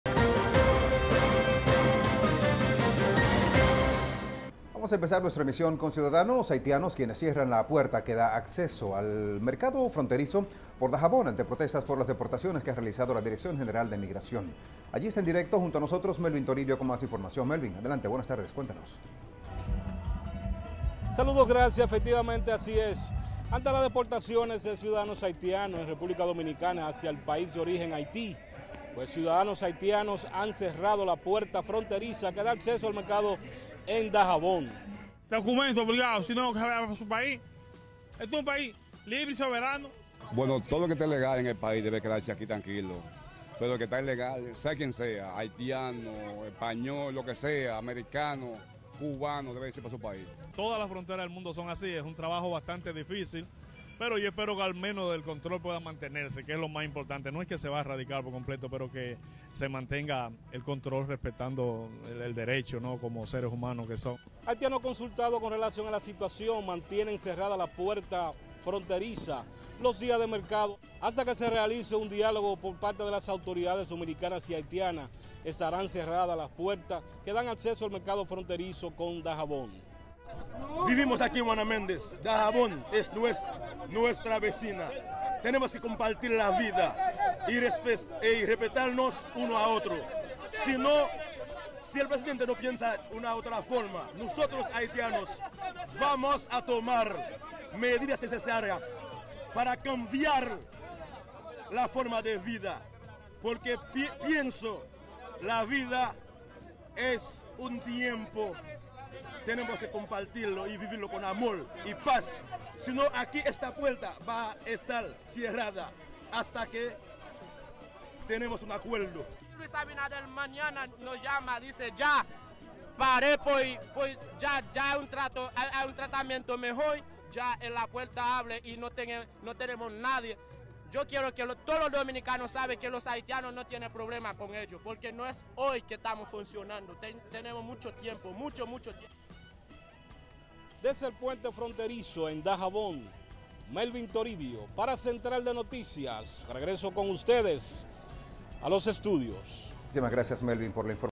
Haitianos cierran puerta de acceso al mercado fronterizo ante protestas por las repatriaciones
Periodista: Melvin Toribio